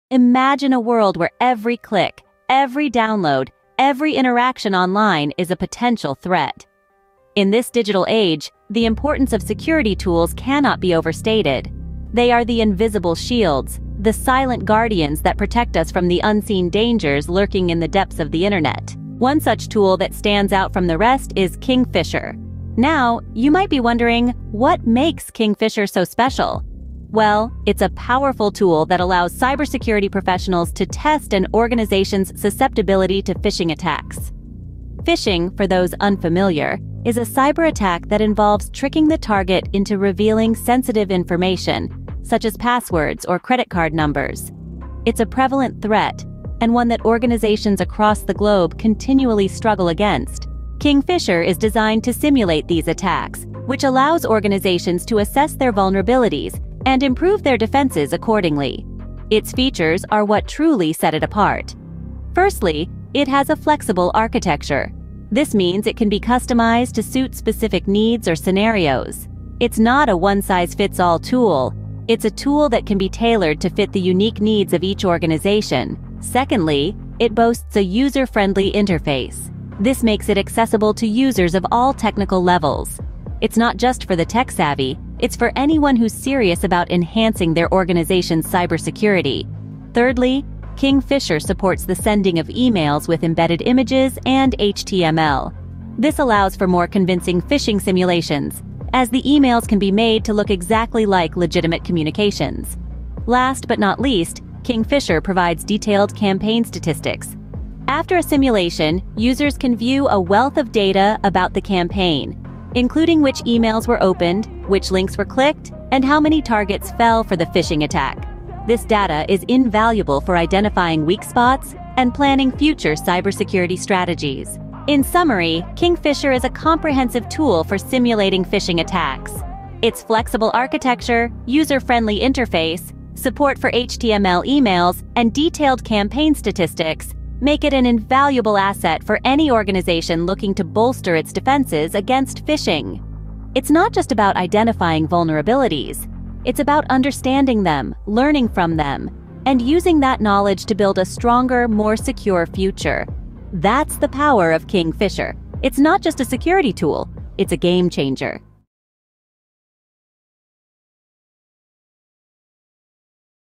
What is King Phisher?
King Phisher is a versatile open-source phishing campaign toolkit designed for security professionals and penetration testers to assess the awareness and resilience of organizations against phishing attacks. It enables users to create and execute simulated phishing campaigns with ease, featuring customizable email templates, detailed tracking of user interactions, and comprehensive reporting functionalities.